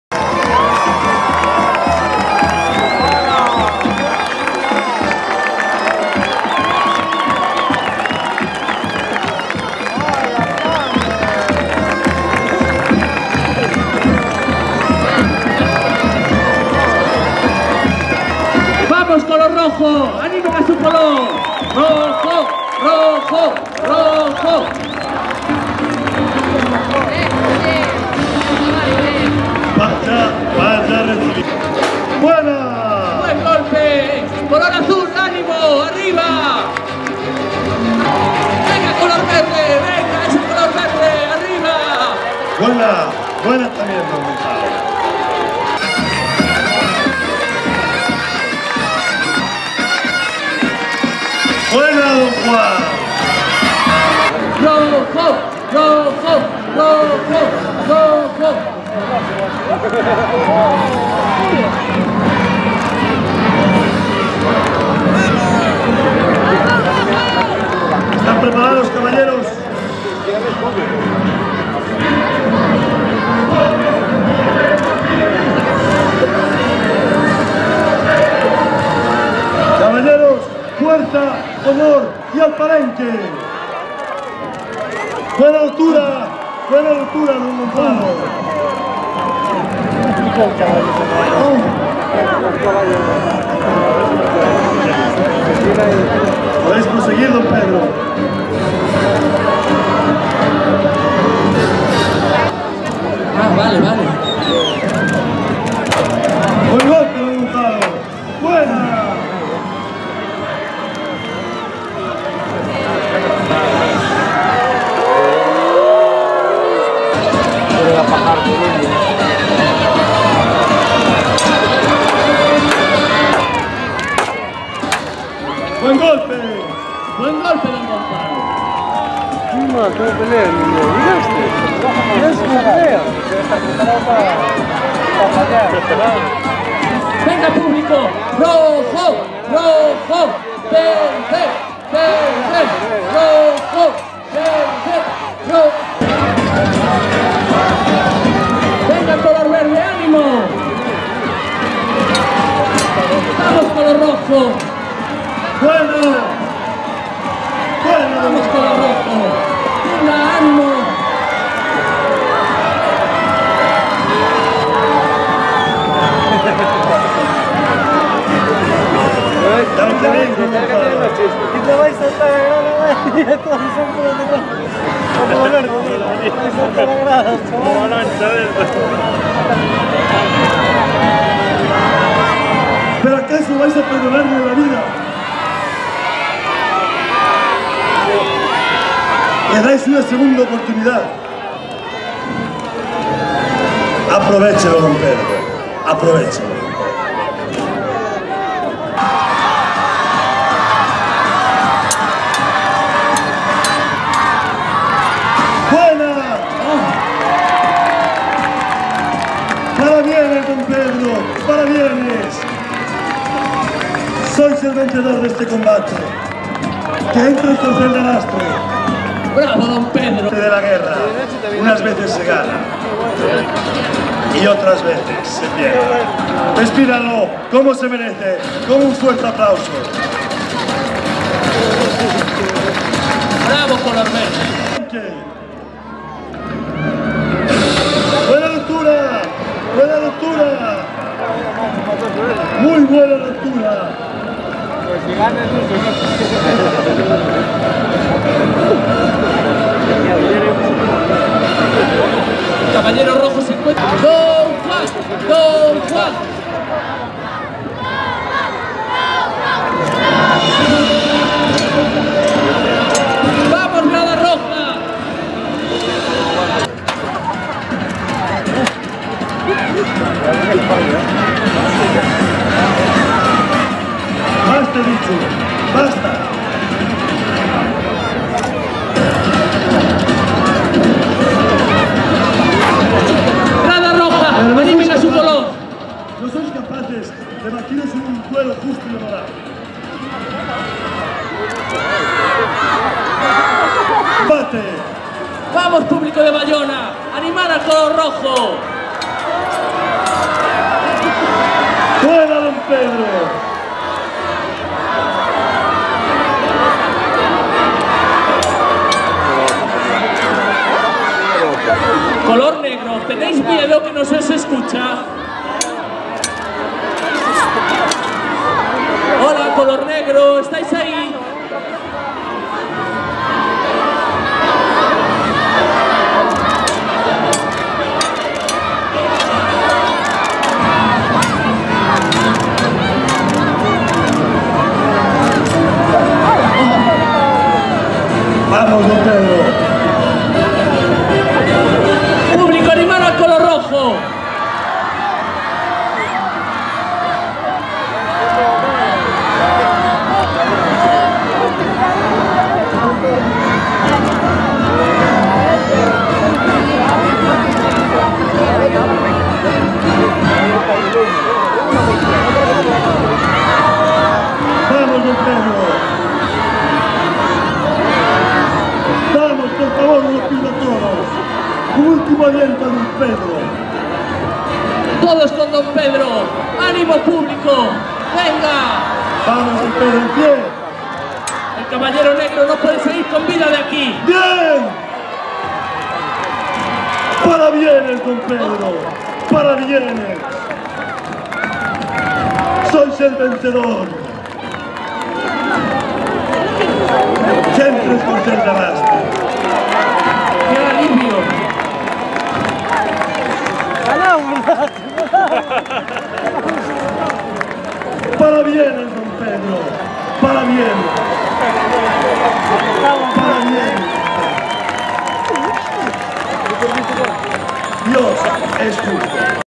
Torneo medieval en la Arribada 2015 de Bayona
Torneo de justa, tiro de venablos y derribo del estafermo en la playa de la Ribeira de Bayona (Galicia, España), con motivo de la fiesta de la Arribada 2015, con la que se conmemora la llegada de la caravela "La Pinta" el 1 de marzo de 1493 a esta localidad, que fue la primera de Europa en recibir la noticia del Descubrimiento de América. El torneo corrió a cargo del Grupo de Especialistas de Hípica Celta.